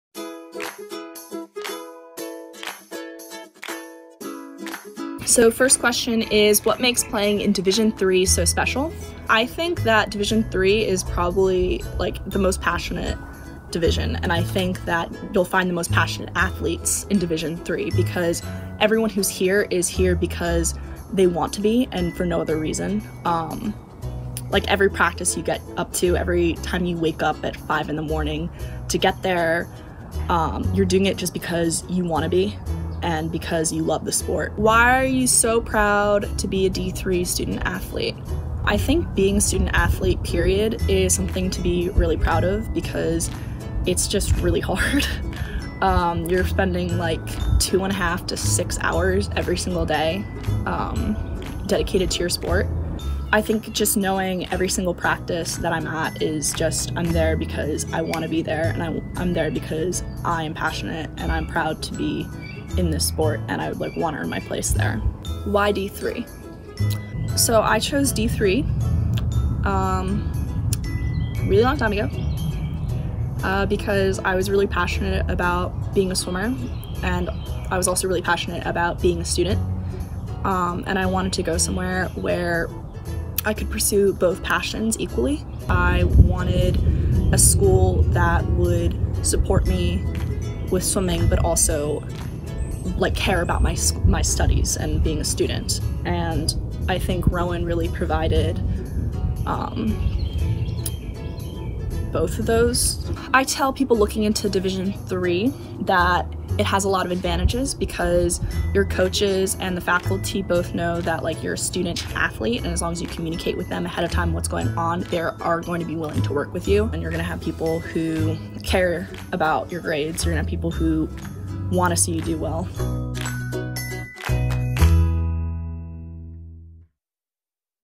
NCAA D3 Week | Claire McKissick | Rowan Athletics
Rowan women's swimmer Claire McKissick, talks about her division three experience and why her decision to come to Rowan was the best she made.